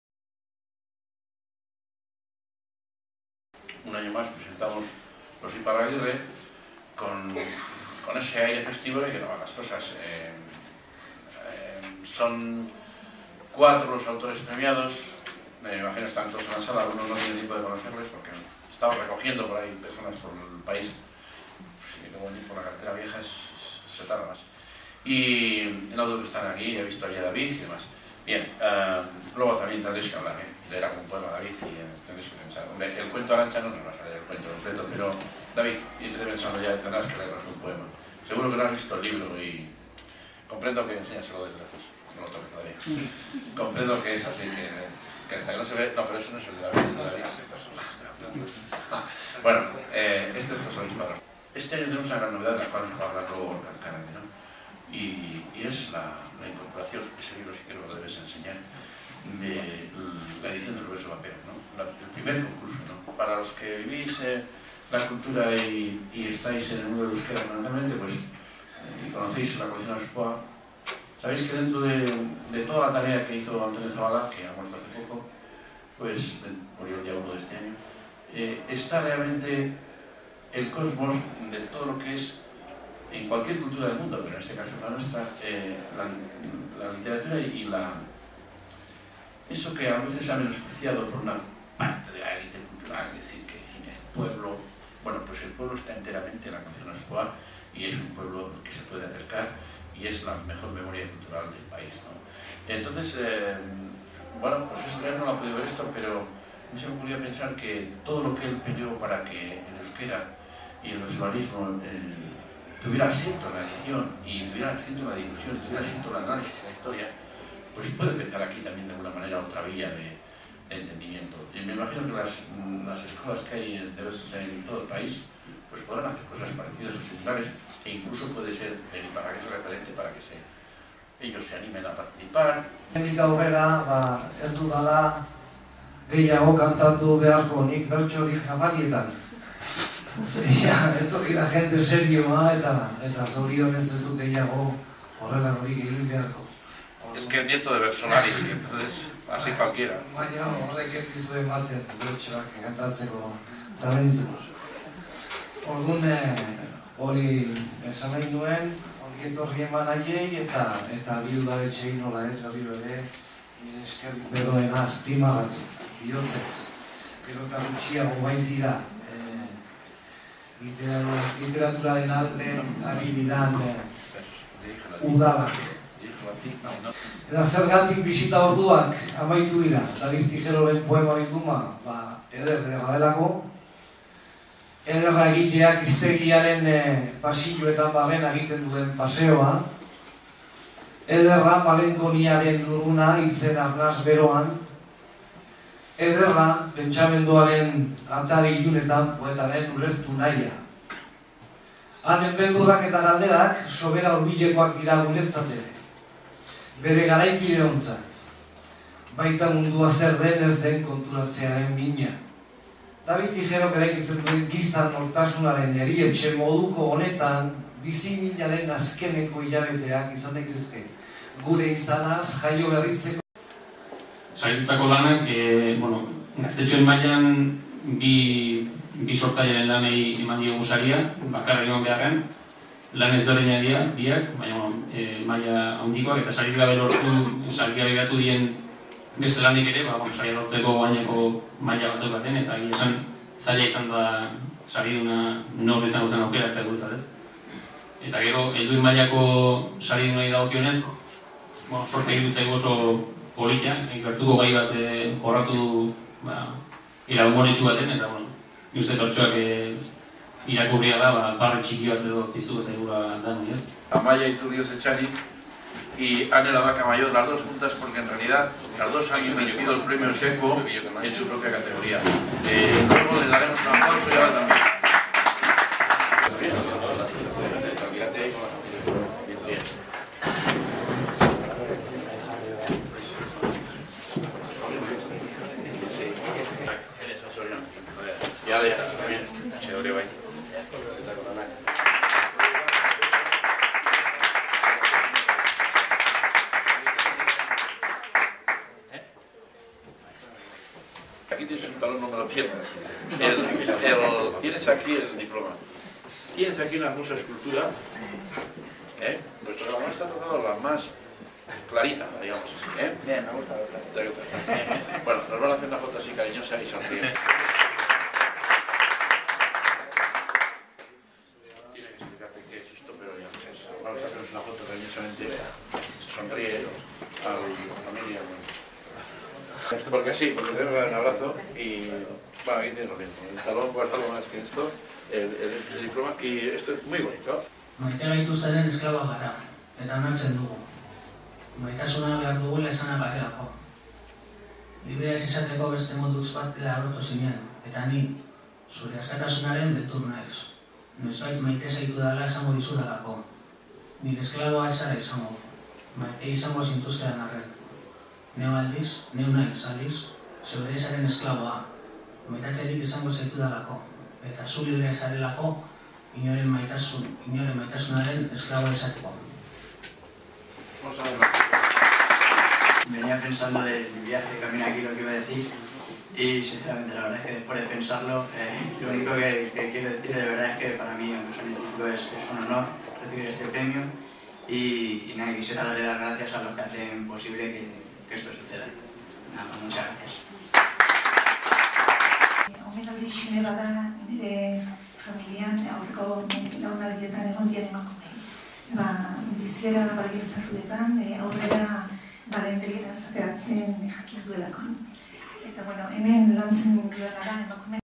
Iparragirre sariak banatu dituzte (2009/02/19)
Amaia Iturriotz urretxuarrak irabazi du 16 urtetik beherakoen artean bertsopaper lehiaketako lehen saria. Gainontzeko sariak bi herrietatik kanpora joan dira.